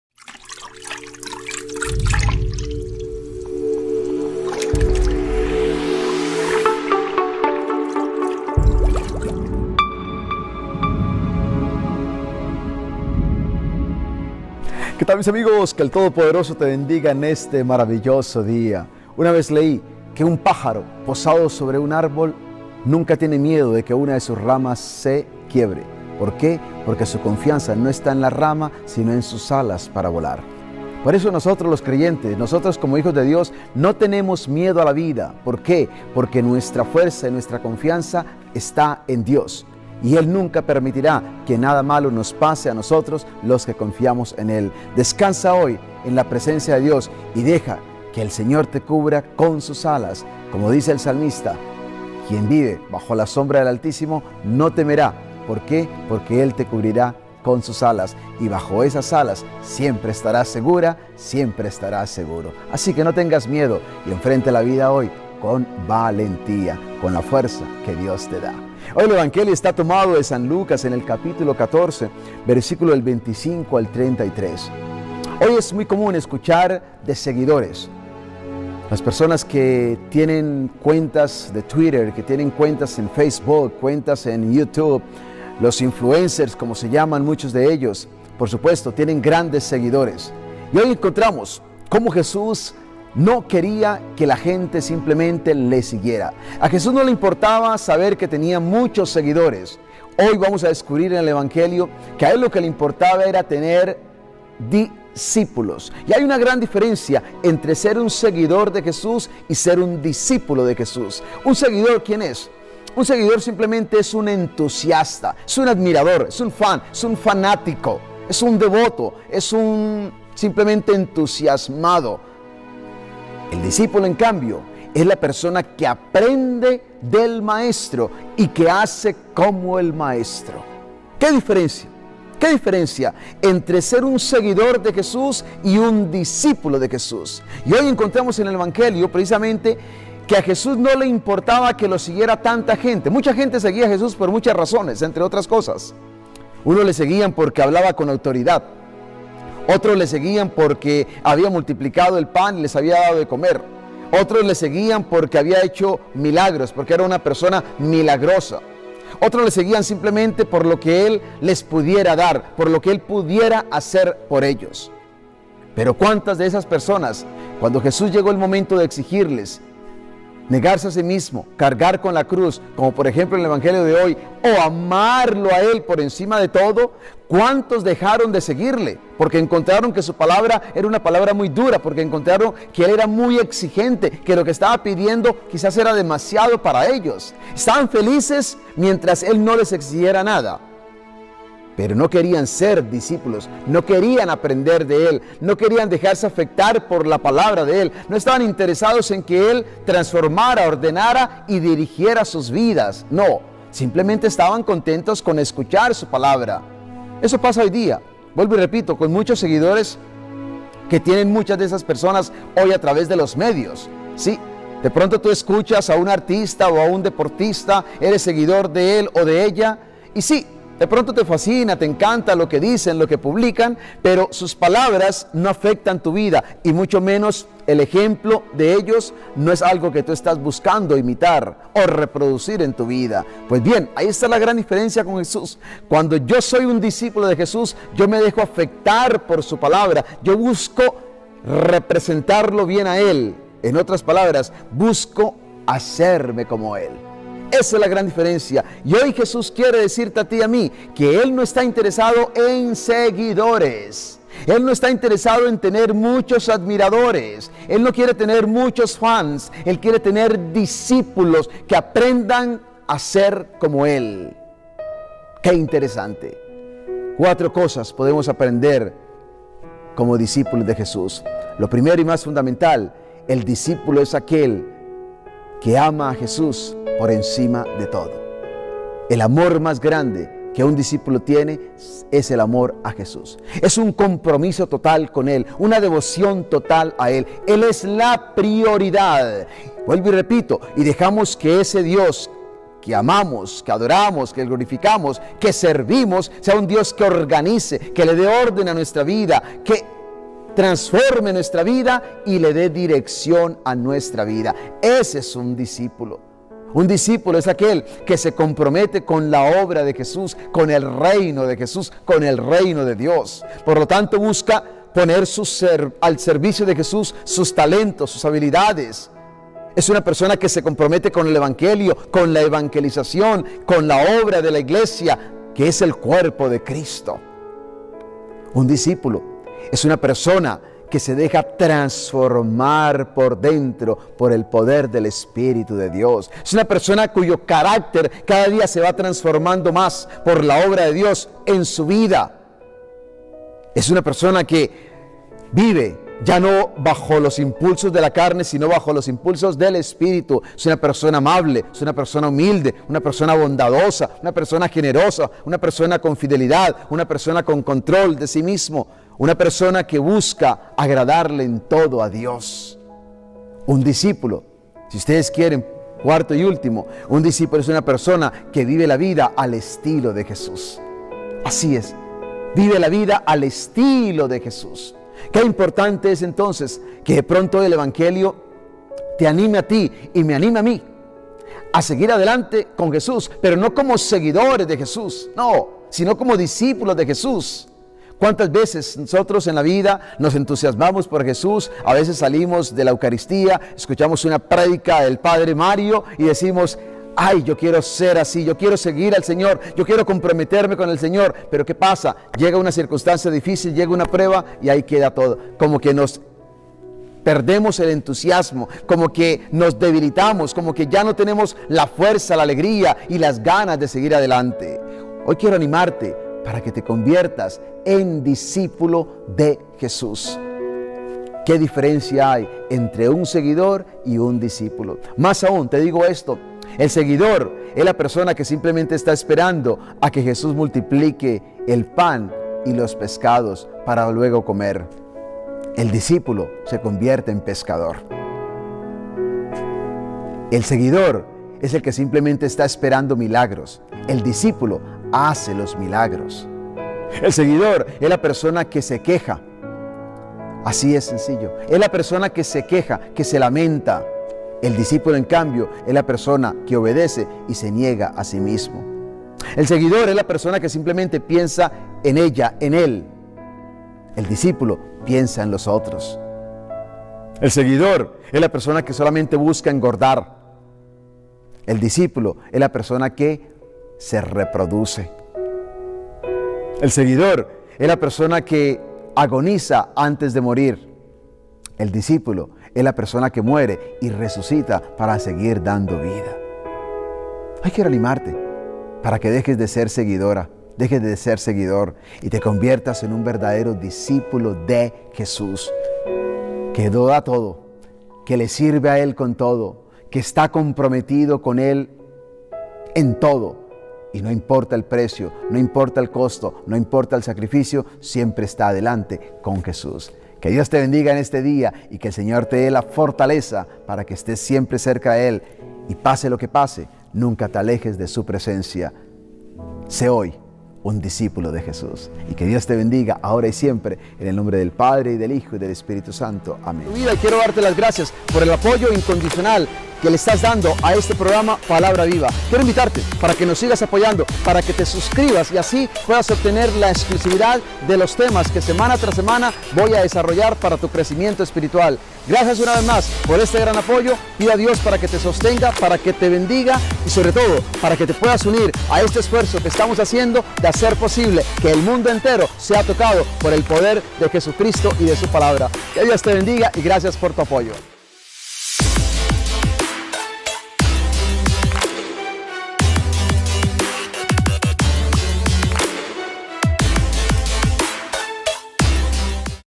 YOUTUBERS VS JESUS-P. Mario Castañeda
El Padre Mario Castañeda, un joven sacerdote colombiano, lleno de energía y pasión por llevar la Palabra de Dios, va más allá del púlpito de su Iglesia St. Rita Catholic Church en Wellington, Florida.
Con su ministerio de evangelización vía internet, "Tu Palabra Hoy", alcanza a muchos fieles con sus interesantes prédicas cargadas de fe y esperanza.